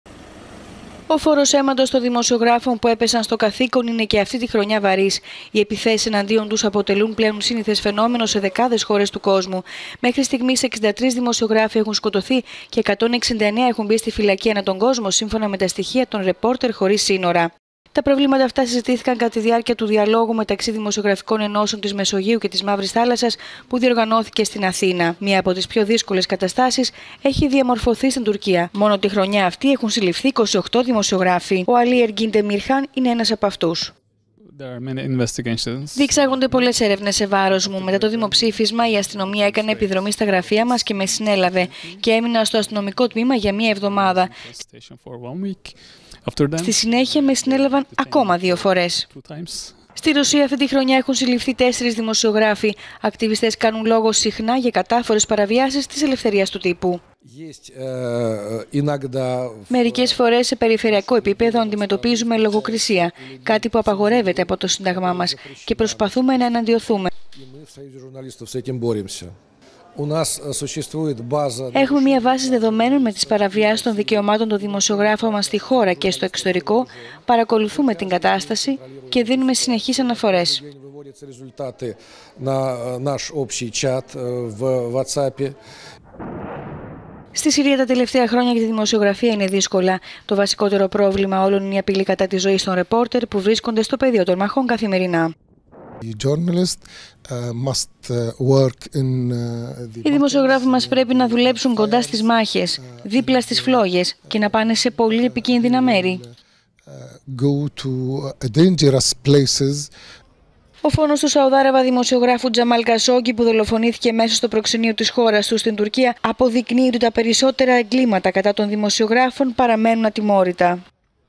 Οι δημοσιογράφοι υπό απειλή παγκοσμίως
Στο euronews μίλησαν εκπρόσωποι των δημοσιογράφων από τη Συρία, τη Ρωσία και την Τουρκία…